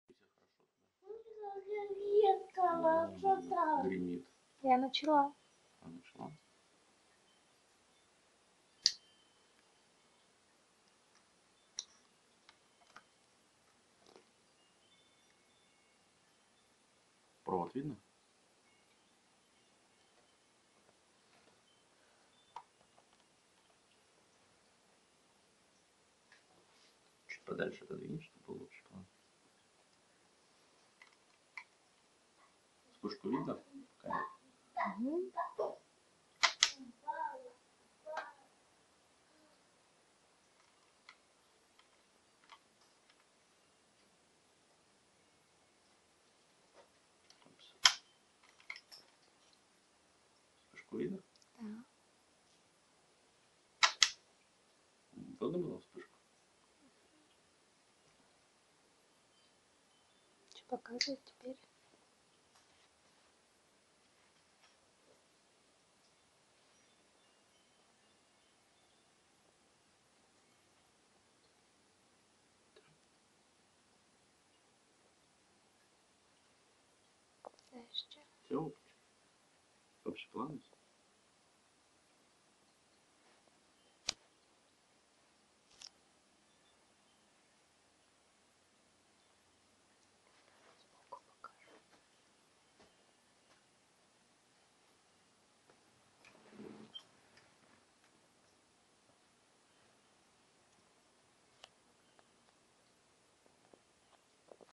Polaroid flashgun #268 - electronic (reusable) flash
The original Polaroid flashgun #268 was designed for disposable flashbulbs. This is my attempt of retrofit it with modern electronic flash.